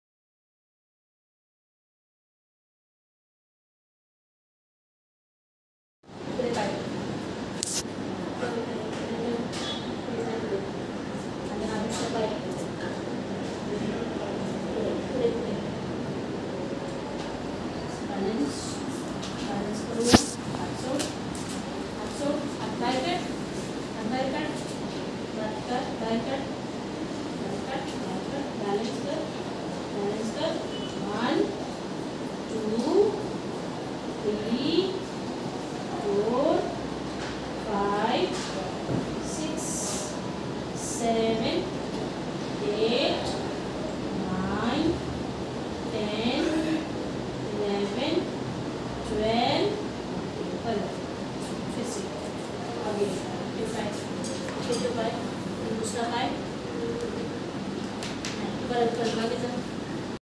Exercises For Special Children.. Balancing
The exercise for ARNAV, a special child at SBP center for PWID .

Aim- Balance and hip muscle strengthening

While he was standing with one foot in front of other on a straight track in heel toe manner, he is expected to  maintain the position without support  which will help him for  strengthening of leg muscles.

Goal is that he should be able to do "lunges" in future.